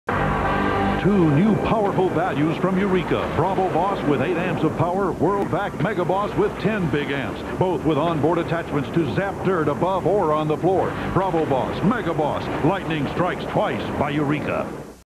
Eureka Vacuum Cleaners - Commercial - Mega Boss - Bravo Boss - MegaBoss - BravoBoss (1993)
Transferred from a VHS recording of "Distant Cousins"  on CBS's Tuesday Night Movie. Recording was made on Tuesday September 14, 1993